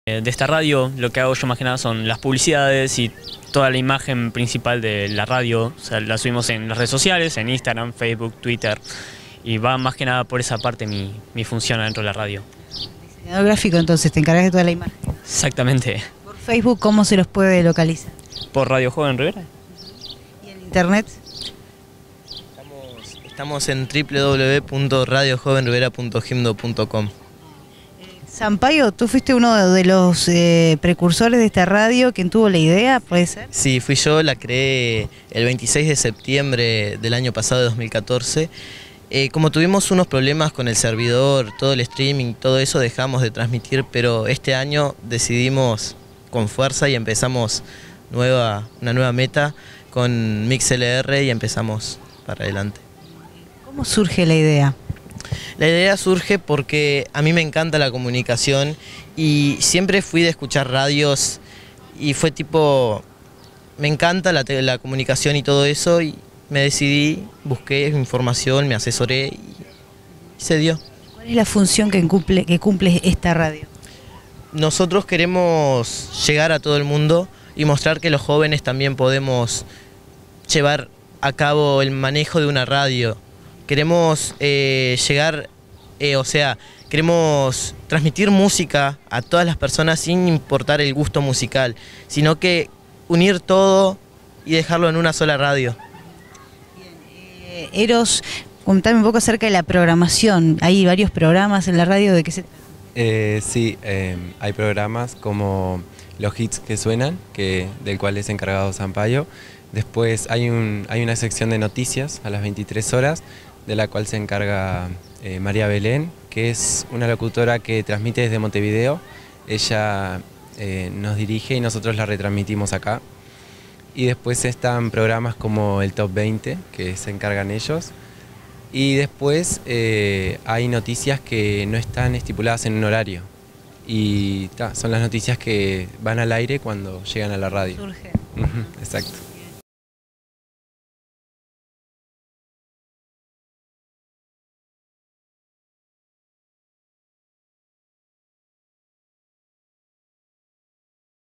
Juan Pablo Bueno, Carlos Sampaio - Integrantes de Radio Joven Rivera
Un grupo de estudiantes del Liceo Nº 1 y de la Escuela Técnica de Rivera han fundado una radio online denominada Radio Joven Rivera. La misma cuenta con una programación dirigida al público joven y puede escucharse a través de TELEDIARIO dialogó con los protagonistas.